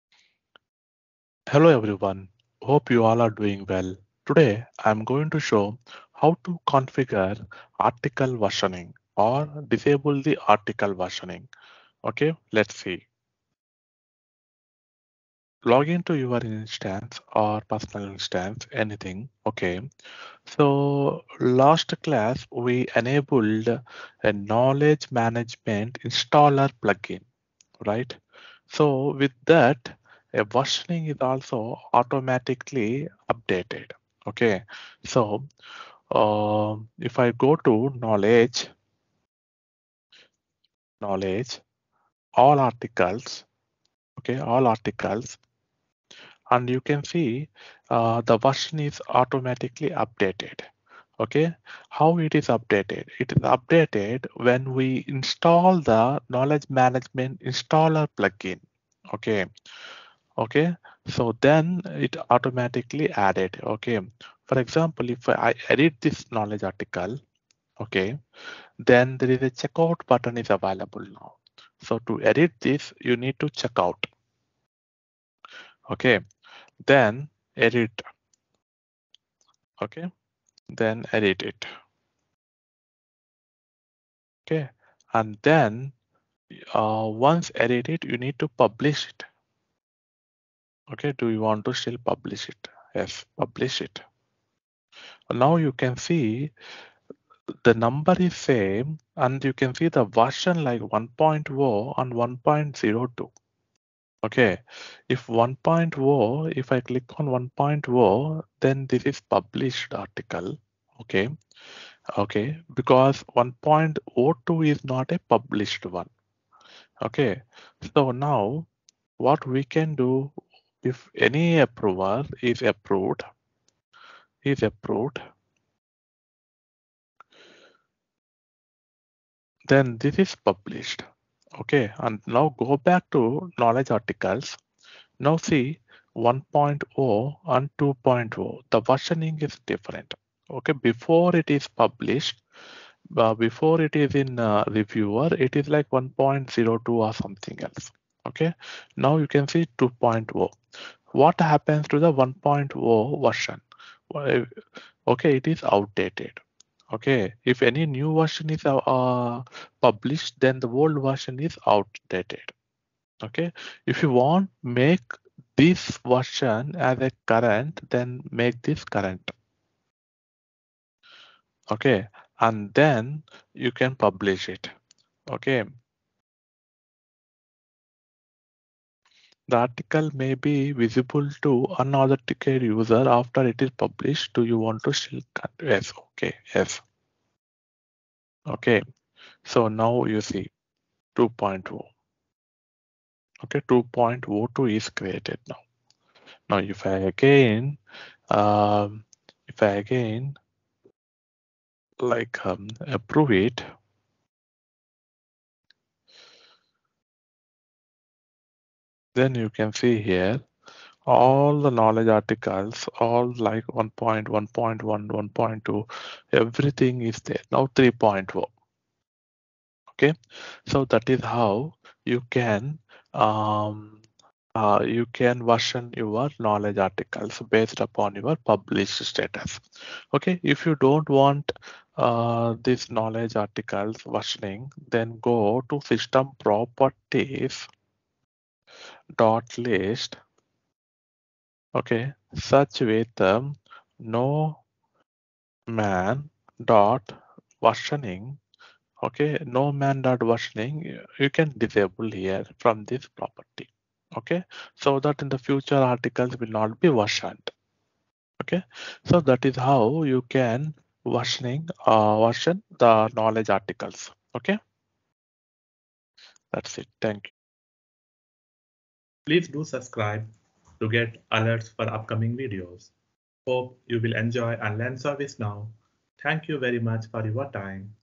ServiceNow Knowledge Management | how to configure versioning on knowledge articles in ServiceNow
Knowledge Management Tutorial with practical demo.

please go the below links for each session

KB Session 8 : TBA
KB Session 9 : TBA